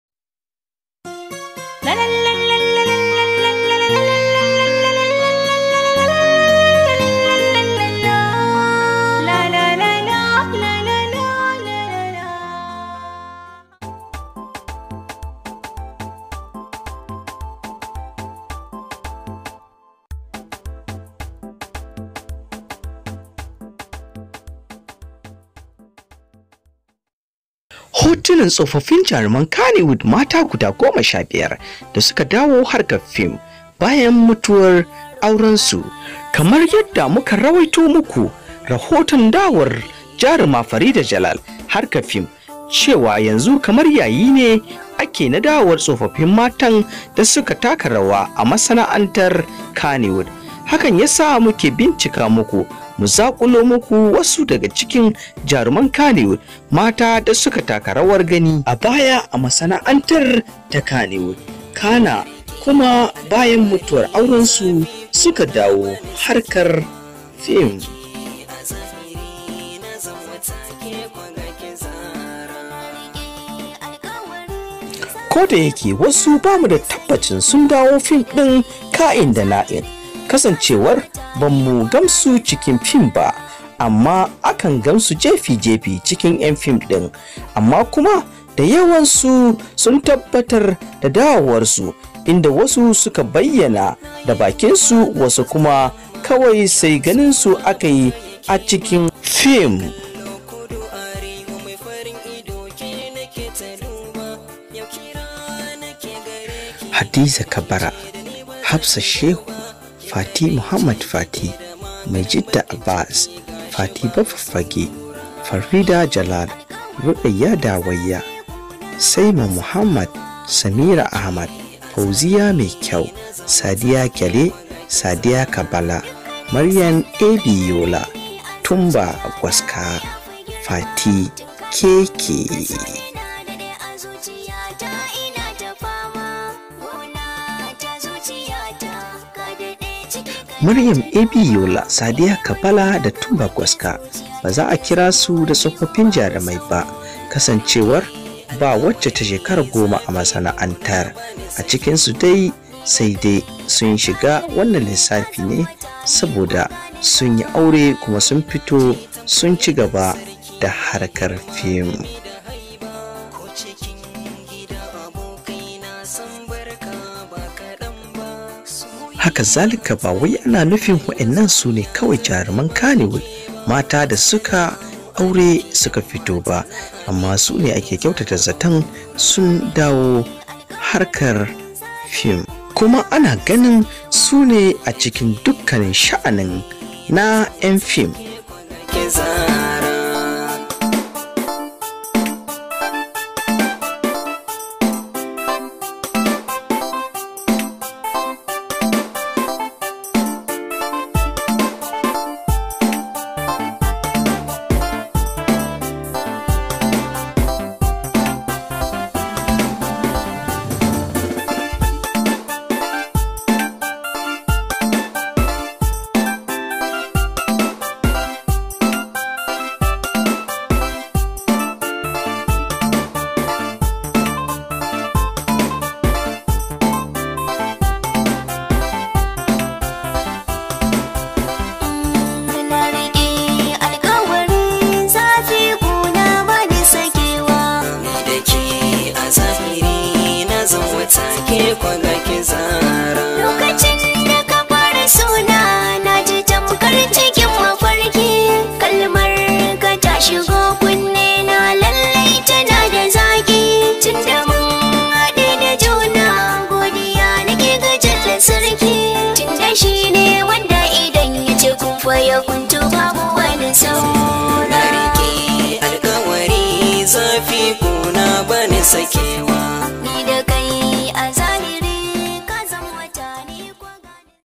Tsofaffin matan kannywood guda 15 da suka dawo masana'antar bayan mutuwar auren gasu kamar haka.....
Matan kannywood da suka dawo film bayan mutuwar aurensu!!!